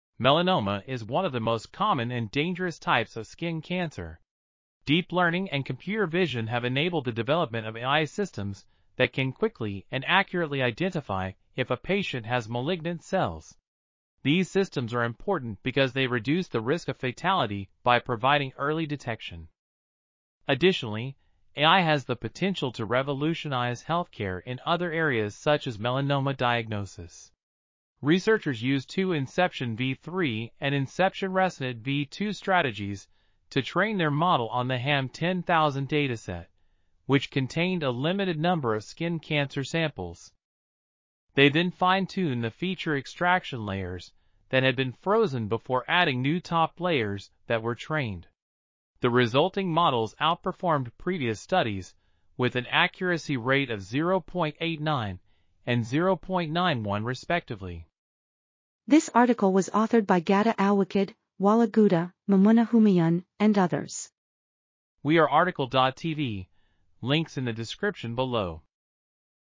Diagnosing Melanomas in Dermoscopy Images Using Deep Learning | RTCL.TV
### Keywords ###

### Article Attribution ###
Title: Diagnosing Melanomas in Dermoscopy Images Using Deep Learning
Authors: Ghadah Alwakid, Walaa Gouda, Mamoona Humayun ,and N. Z Jhanjhi
Publisher: MDPI AG
DOI: 10.3390/diagnostics13101815

### Video Timestamps ###
0:00:00 - Summary
0:00:58 - Title
0:01:04 - Outro
0:01:09 - End